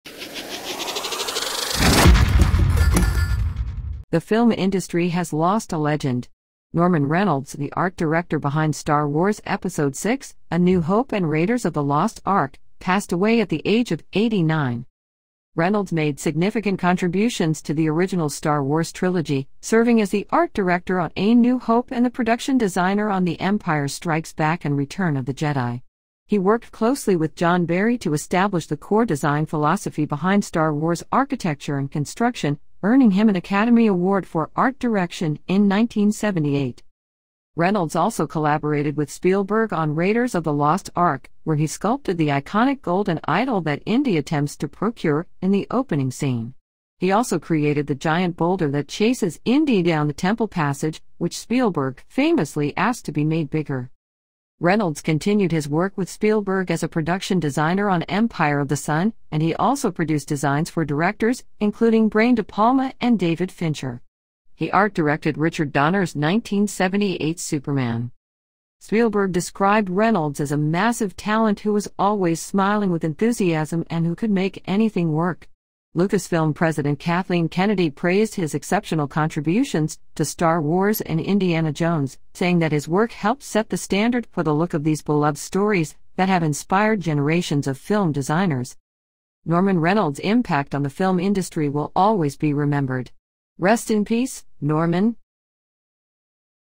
Remembering Norman Reynolds: The Art Director behind Star Wars and Raiders of the Lost Ark
In this video, we pay tribute to the late Norman Reynolds, the renowned art director who contributed to the iconic films of Star Wars and Raiders of Lost Ark. From sculpting the golden idol in the opening scene of Raiders to creating the look of the architecture in Star Wars, Reynolds made a significant impact on the film industry. Join us as we celebrate his life and his remarkable career.